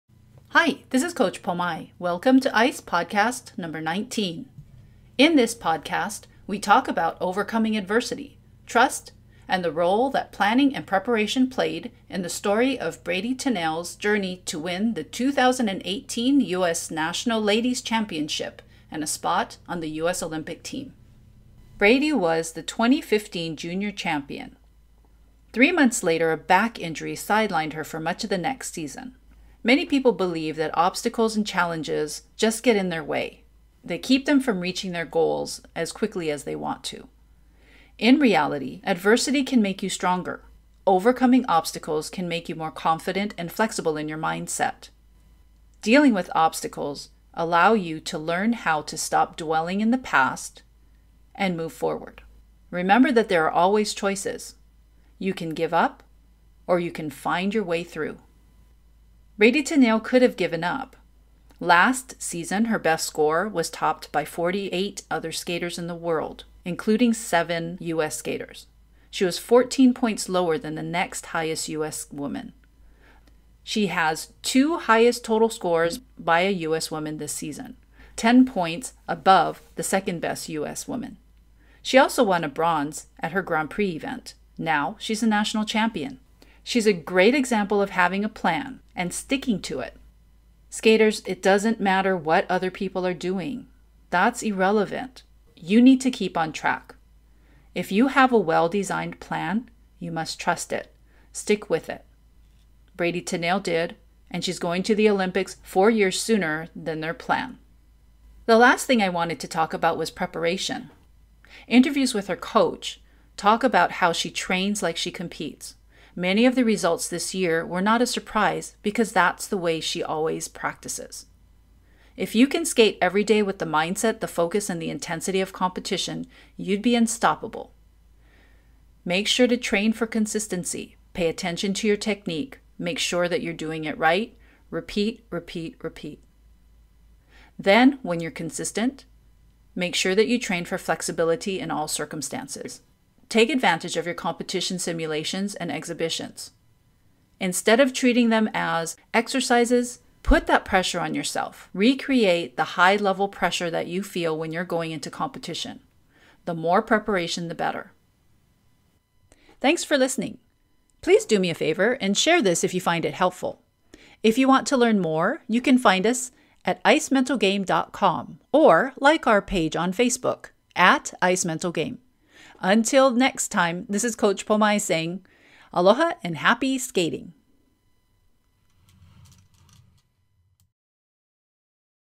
ICEPod Episode 19: Mental Game in Action: US Champion, Bradie Tennell
In this podcast we talk about overcoming adversity, trust and the role that planning and preparation played in the story of Bradie Tennell’s journey to win the 2018 US National Ladies Championship and a spot on the US Olympic Team.